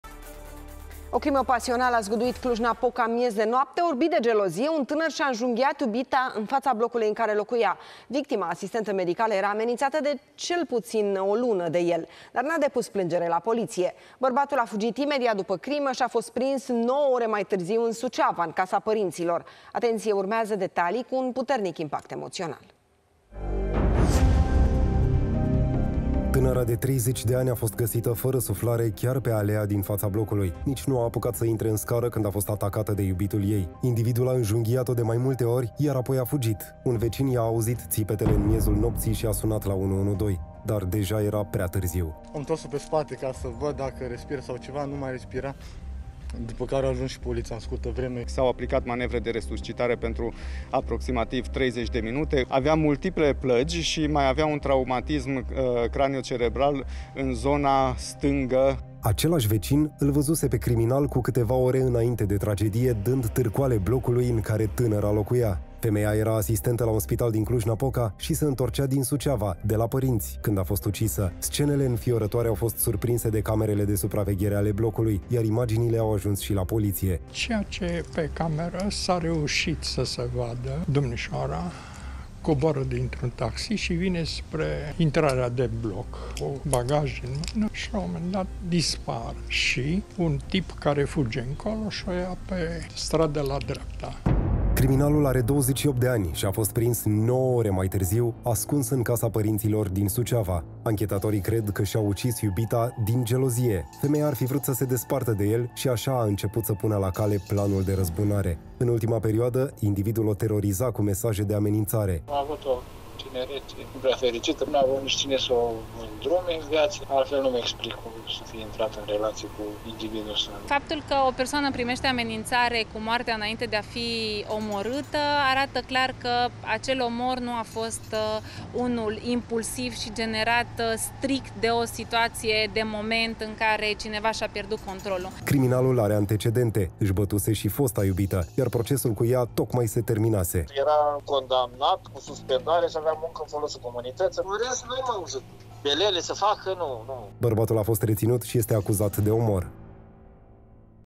Tânără asistentă ucisă de iubitul gelos, la Cluj. Criminalul a fost găsit de polițiști
O crimă pasională a zguduit Cluj-Napoca în miez de noapte. Orbit de gelozie, un tânăr şi-a înjunghiat iubita chiar în faţa blocului în care locuia. Victima, asistentă medicală, era ameninţată de el de cel puţin o lună. Dar nu a depus plângere la Poliţie! Bărbatul a fugit imediat după crimă şi a fost prins nouă ore mai târziu, în Suceava, în casa părinţilor.